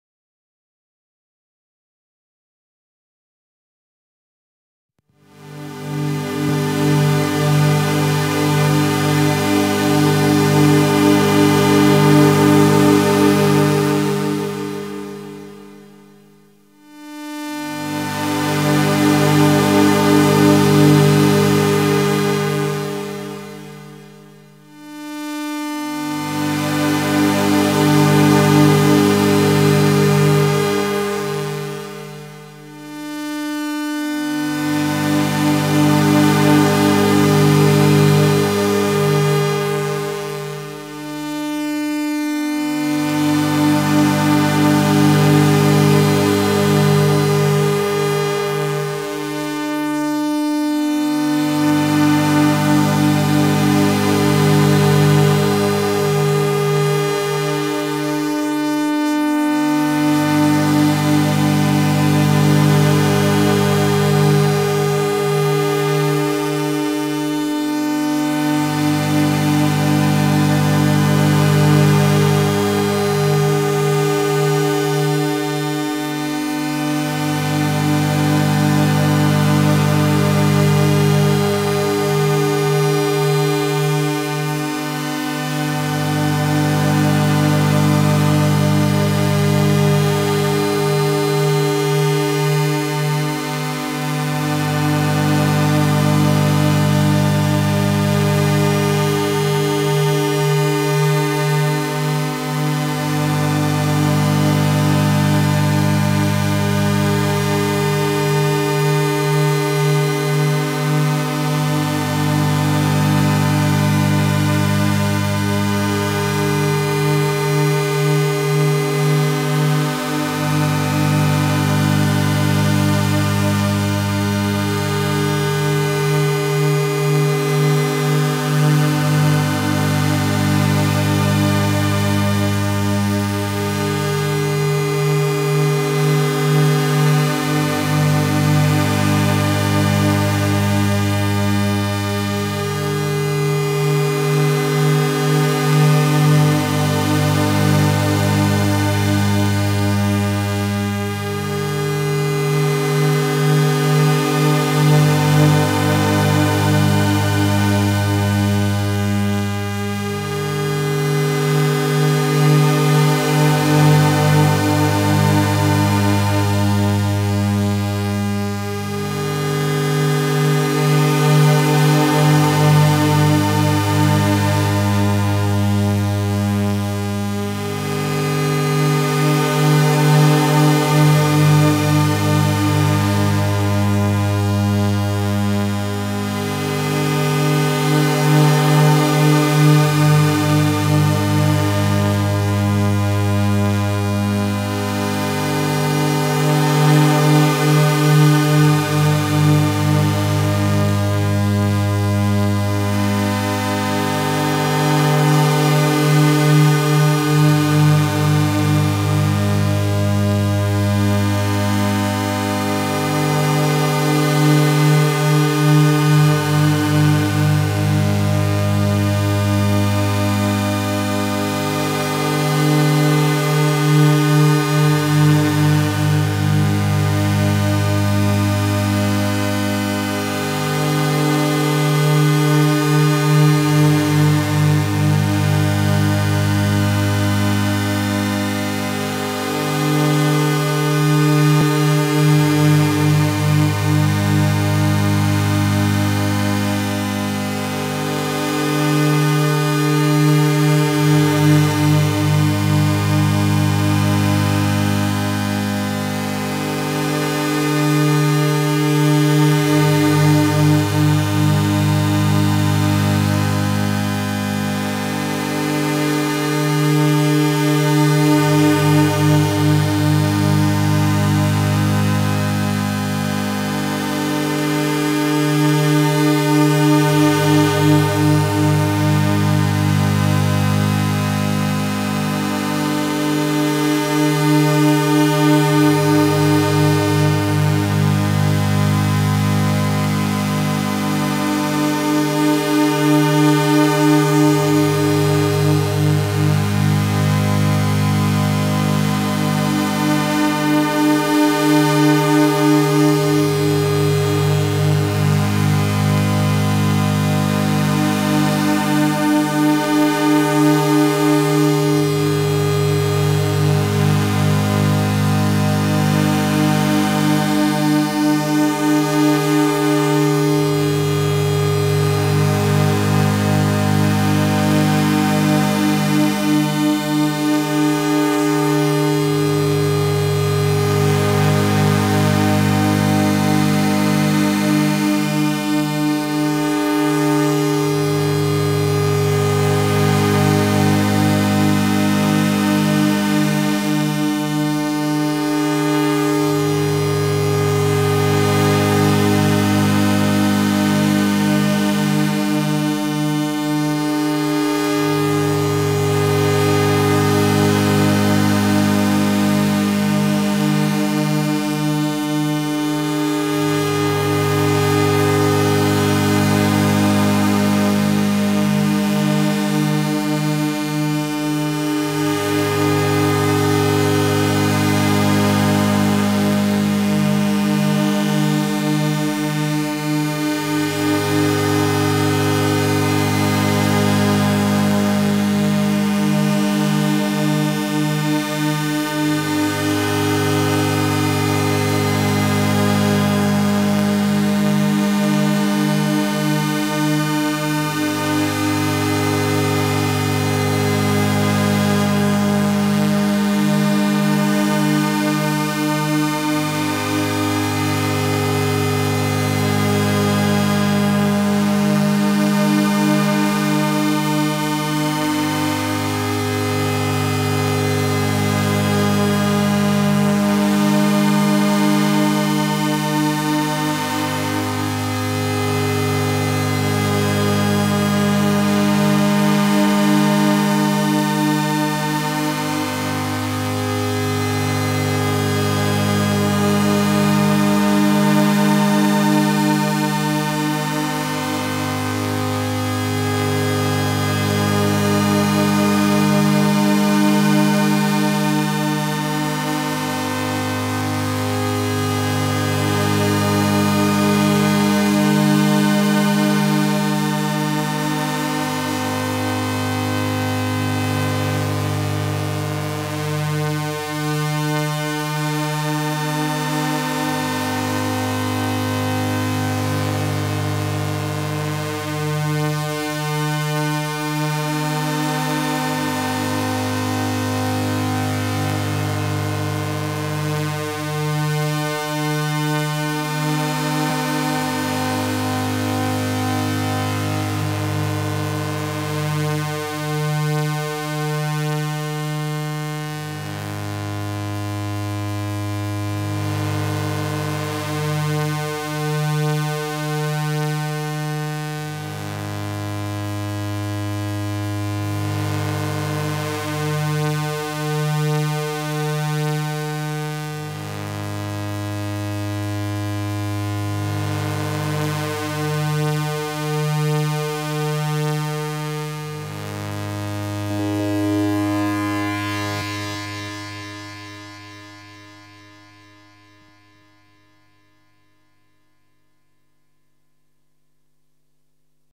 Enneatronic Spectralizer Demo [2012]
The Enneatronic Spectralizer is a 9 oscillator drone box with a hypnotic LED display.